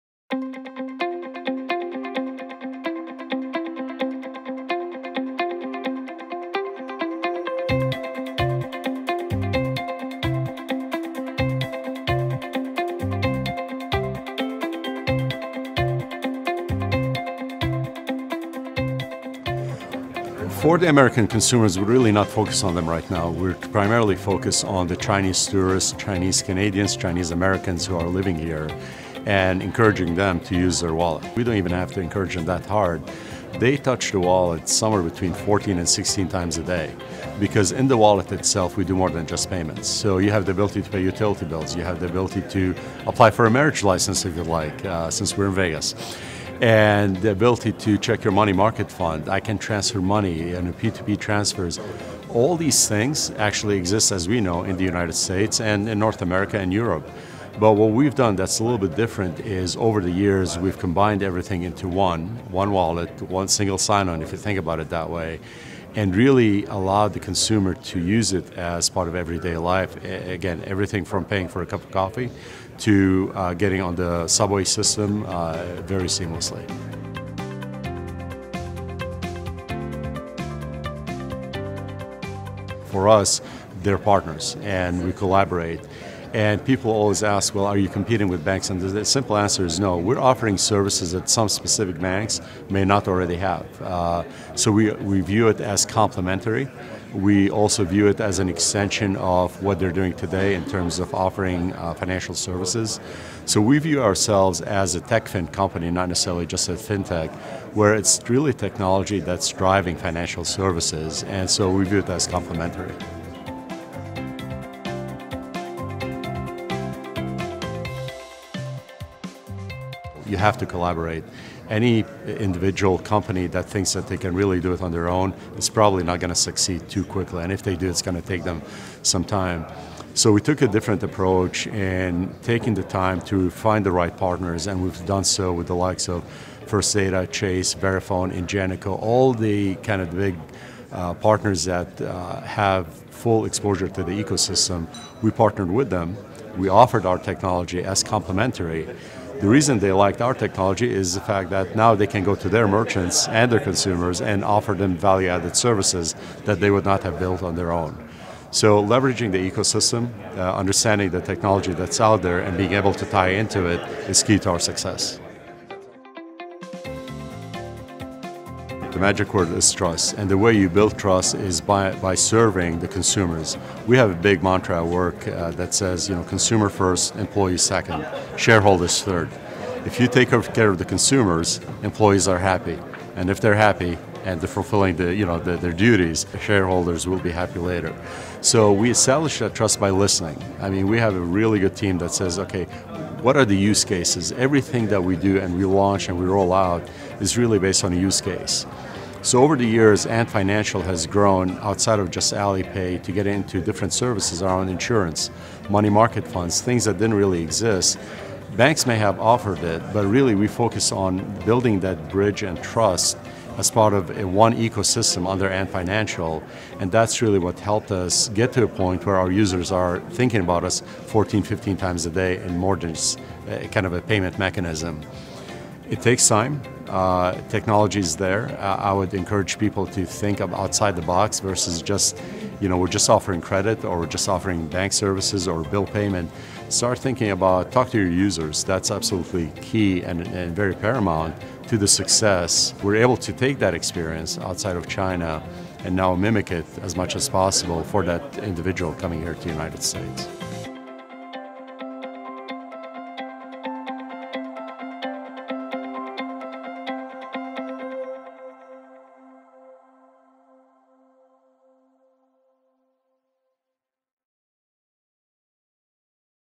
Agent of Change Souheil Badran, Alipay North America, Ant Financial
Check out our discussion with Souheil Badran, President, Alipay Americas, Ant Financial, about the shifting landscape of the financial services industry. This video covers the way AliPay looks at collaboration with banks and third-parties and the importance of trust within the industry.

for details about the World FinTech Report 2018.

Follow WFTR18 on Twitter